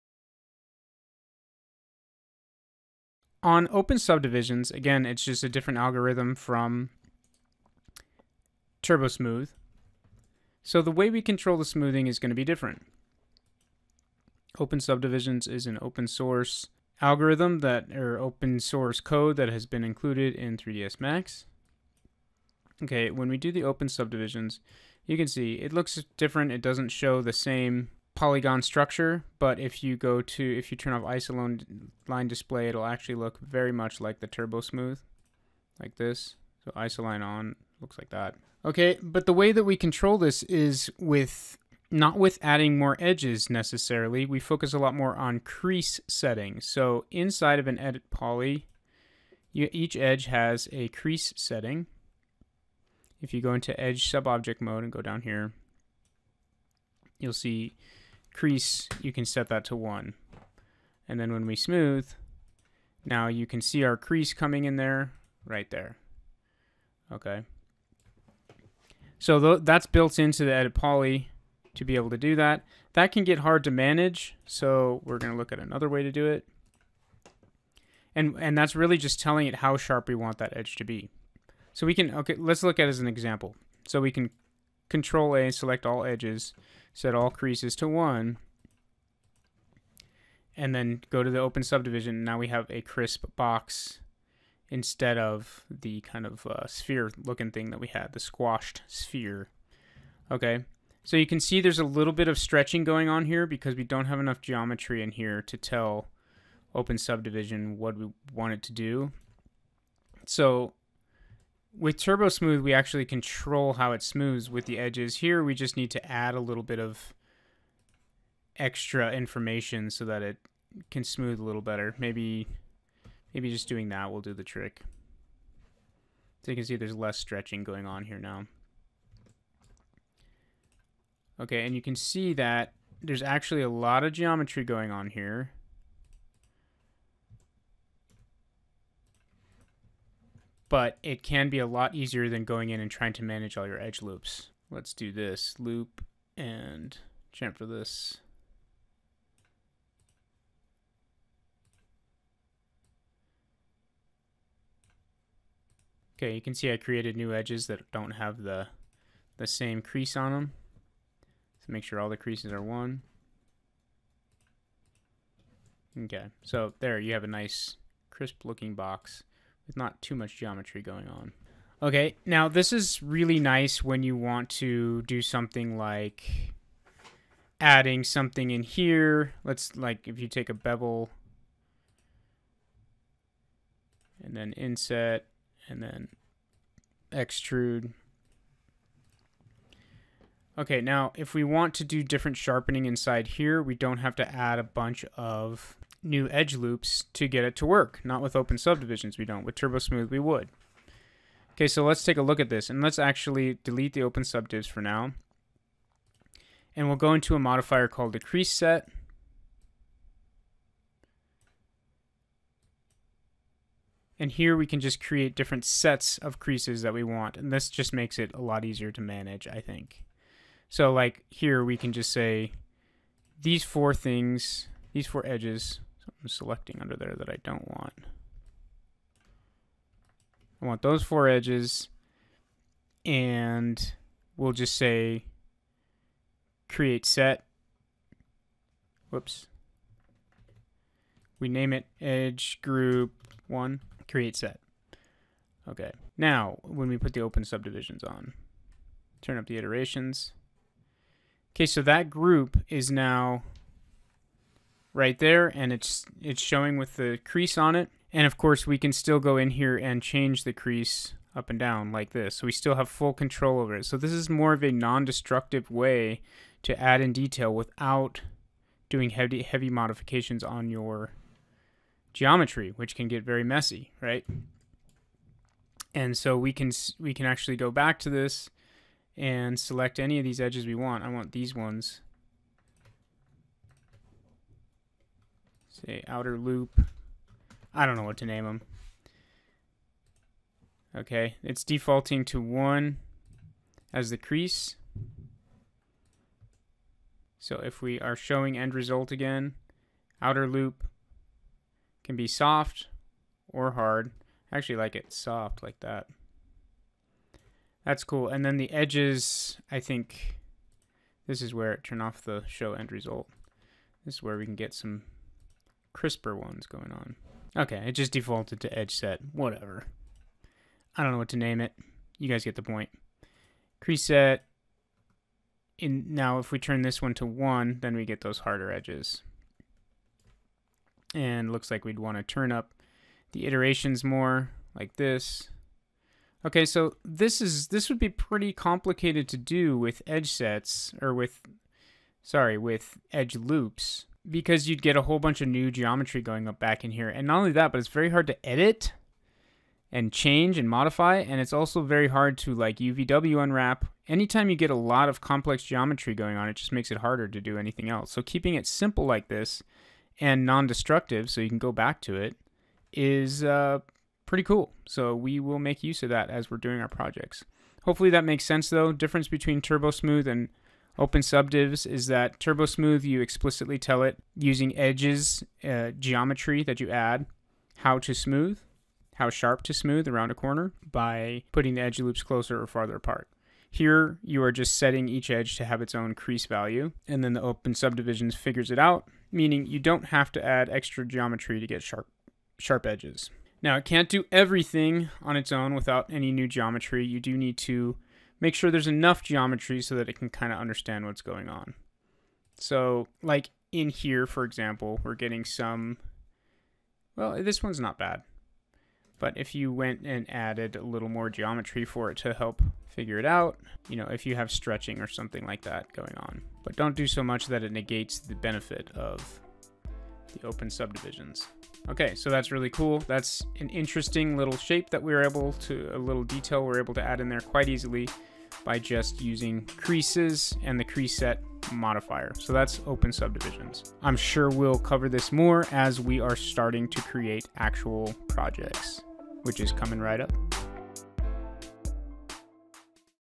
Tutorial: Using OpenSubdivs and Crease Sets in 3ds Max | The Best Way To Smooth Poly Models
Get this poly modeling course, and all other free resources

This tutorial shows the basics of using the opensubdivs modifier in 3ds Max. Opensubdivs is a smoothing modifier that has been added to 3ds Max in more recent releases, and it rivals turbosmooth for the top spot when it comes to smoothing your poly models. In fact, in my opinion it is better. For me, it makes it much easier to handle smoothing in an intuitive and efficient way, and it is for more non-destructive for me. Instead of adding edge loops everywhere to control how sharp or soft a corner is, opensubdivs focuses on using crease sets. This allows the modeler to create hard edges without adding extra geometry to the base model. As you will see in the video, it is fairly easy to keep your model clean, and it is much easier to go back if you want to change something later. This video also demonstrates how to use the open subdivs with a crease set modifier in order to have maximum control when poly modeling

*this is just one lecture from a full professional poly modeling course. Follow the links above to get the full course, which includes full instruction on various poly modeling techniques from beginning to end.